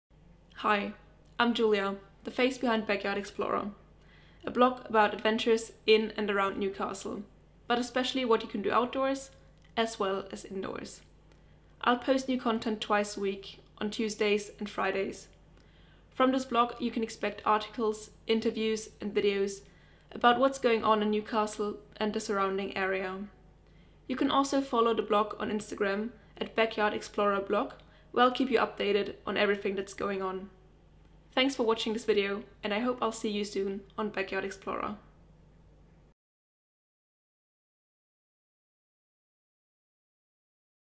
Backyard Explorer: Adventures in and around Newcastle
When we want to go on an adventure or a relaxed holiday we fly to Asia, Spain or Italy. But there are many things we can do at home. Join me and find out more about what you can explore in and around Newcastle.